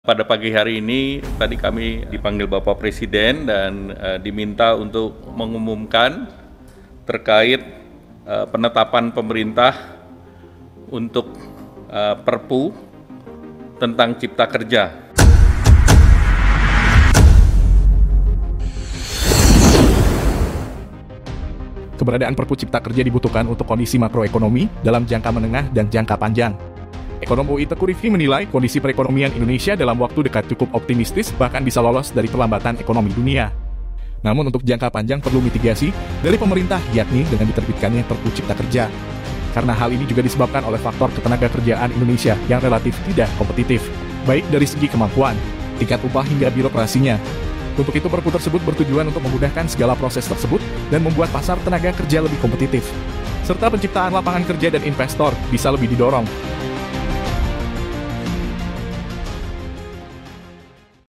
Tujuan Mulia Perppu Cipta Kerja
Keberadaan Perppu Cipta Kerja dibutuhkan untuk kondisi makro ekonomi, dalam jangka menengah dan jangka panjang.

Ekonom UI Teuku Riefky menilai, kondisi perekonomian Indonesia dalam waktu dekat cukup optimistis, bahkan bisa lolos dari perlambatan ekonomi dunia.

Namun untuk jangka panjang, perlu mitigasi dari pemerintah, yakni dengan diterbitkannya Perppu Cipta Kerja.

Karena hal ini juga disebabkan oleh faktor ketenagakerjaan Indonesia yang relatif tidak kompetitif, baik dari segi kemampuan, tingkat upah hingga birokrasinya.

Untuk itu, Perppu tersebut bertujuan untuk memudahkan segala proses tersebut, dan membuat pasar tenaga kerja lebih kompetitif.

Serta penciptaan lapangan kerja dan investor bisa lebih didorong.